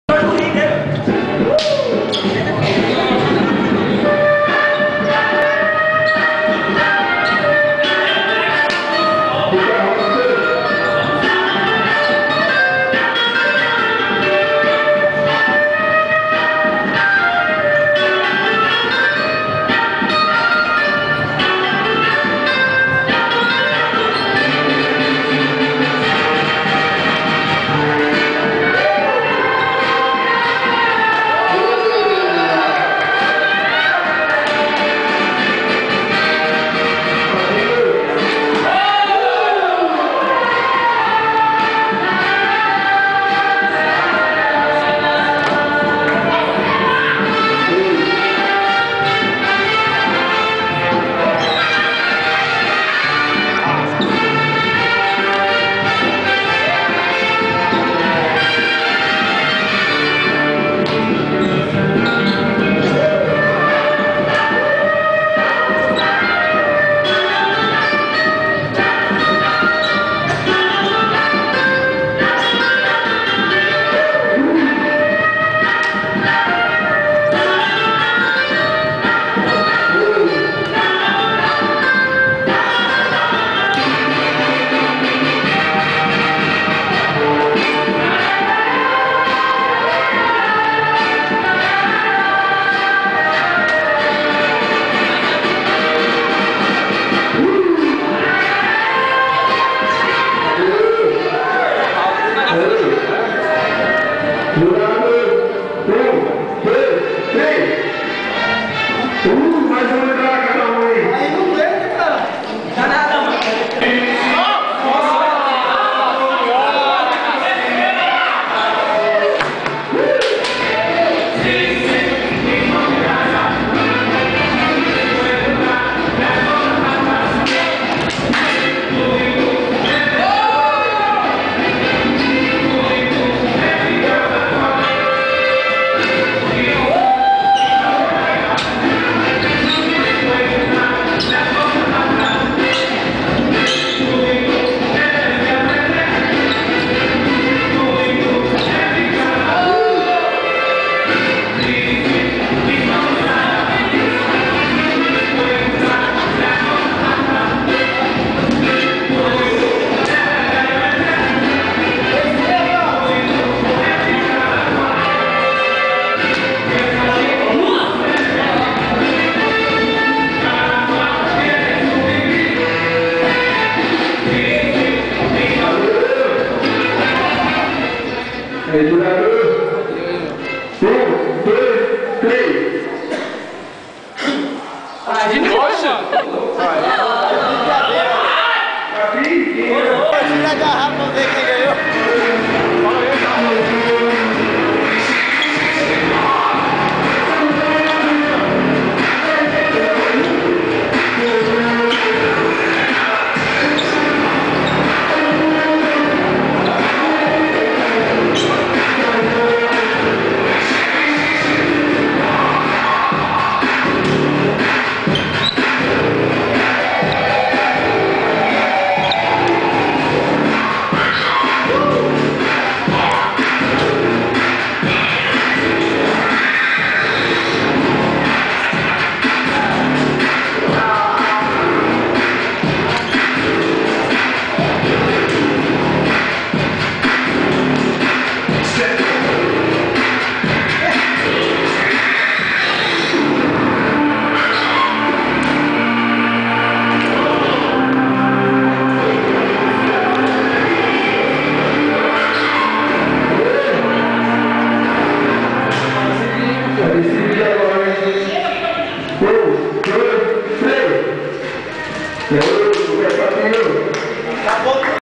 Interior Battle 2011 Tatuí - 8ª Batalha: Robson (DDR CRew) vs Fabinho (Lord's Crew)
Interior Battle 2011 Tatuí - 8ª Batalha de b.boys:  Robson (DDR CRew) vs Fabinho (Lord's Crew)